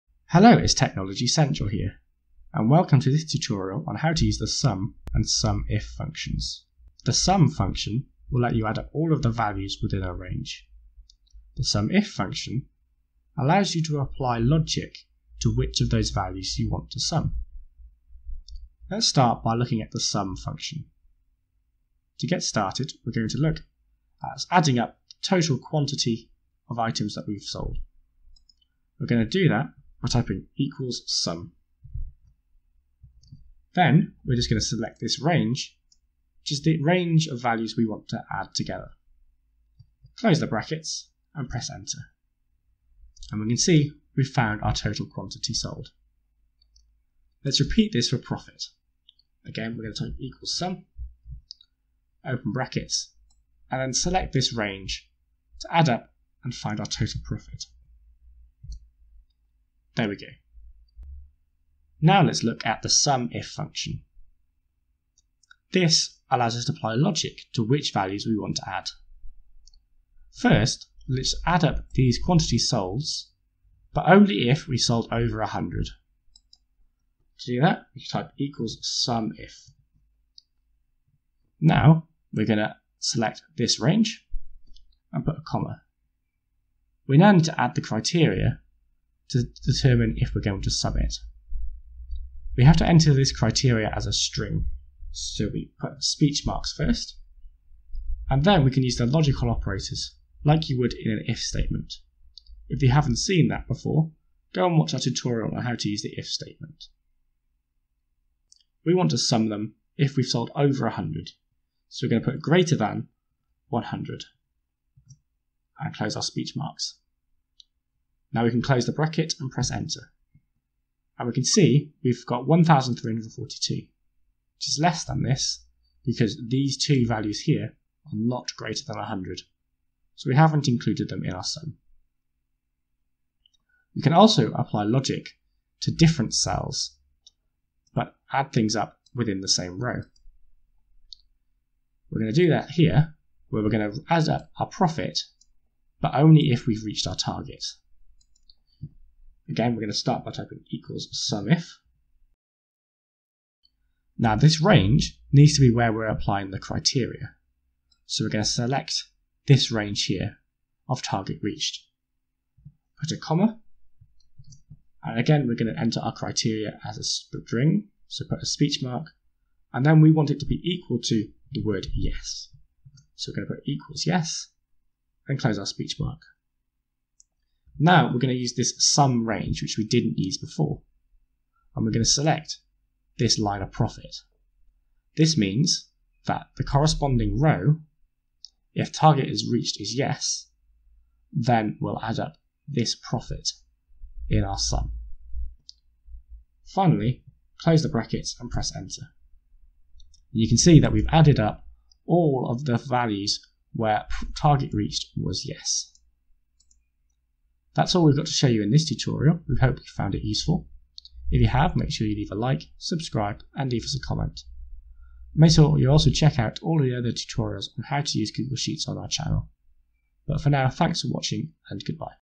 Google Sheets: Sum and Sumif Functions
In this tutorial we show you how to use the SUM and SUMIF functions in Google Sheets. The SUM function is used to add all the values in a range of data. The SUMIF function lets you apply logic to decide which values to add. We show you an example of using the SUM function, using the SUMIF function with the condition being on the same set of data and the SUMIF function with the condition being on data in a different column.